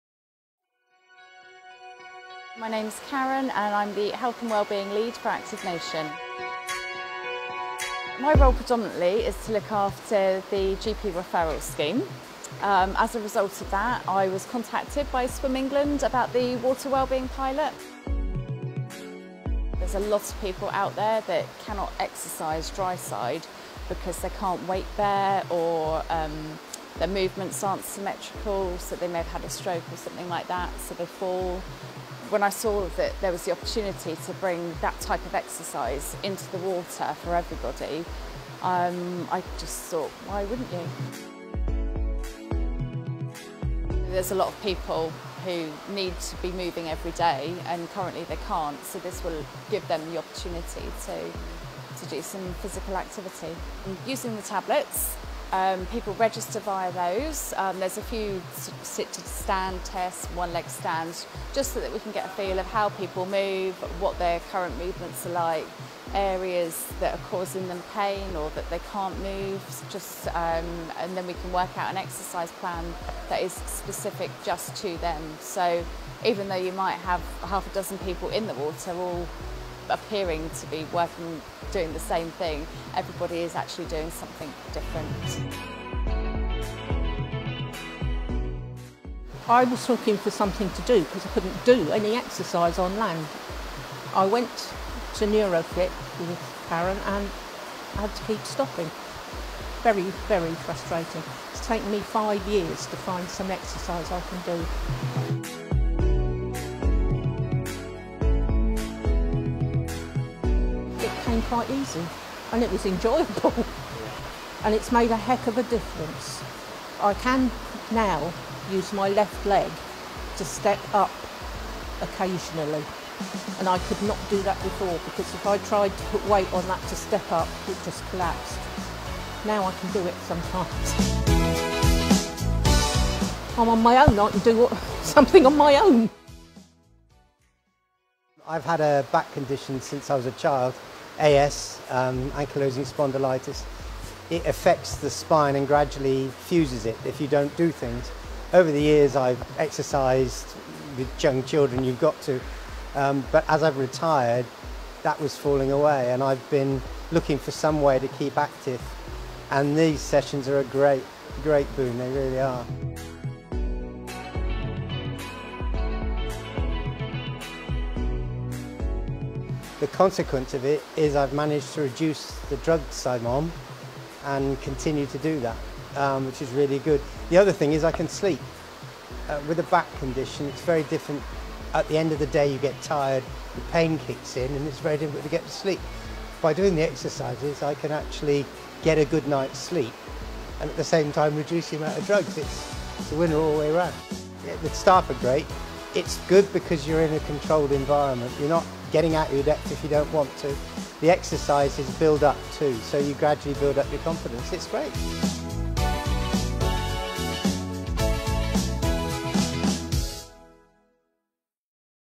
Hydro Aqua
Our Hydro Aqua sessions, which form part of our Swim England Water Wellbeing pilot, are the perfect way to make one small change to your week if you're limited by a long term health condition.